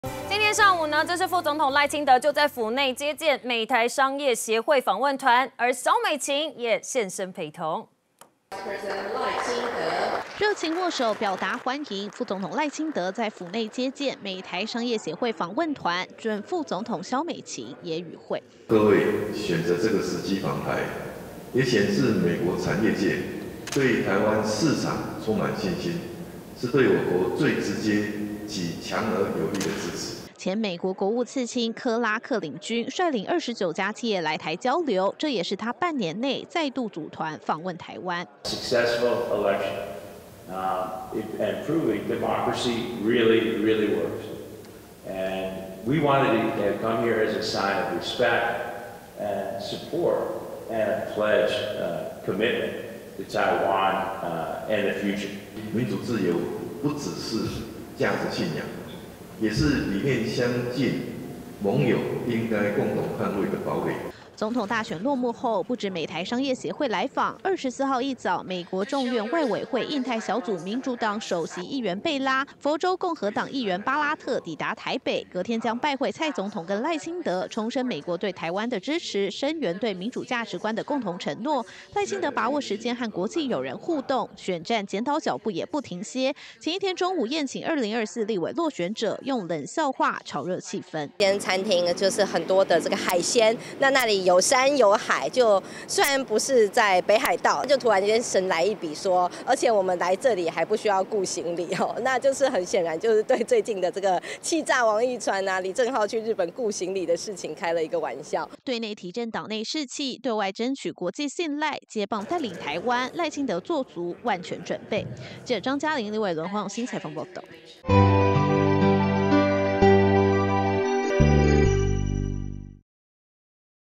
接見美台商業協會訪問團！ 賴清德：民主是價值信仰
2024總統大選落幕，國際友人相繼來訪！ 今（24）日上午副總統賴清德在府內接見美台商業協會訪問團，準副總統蕭美琴也現身。率隊的前美國國務次卿柯拉克當面向賴蕭表達祝賀，更重申美國對台灣的堅定承諾。與此同時，美國國會眾議院「台灣連線」2名眾議員也在今日抵達台灣。

下載【年代電視】APP：
iOS平台：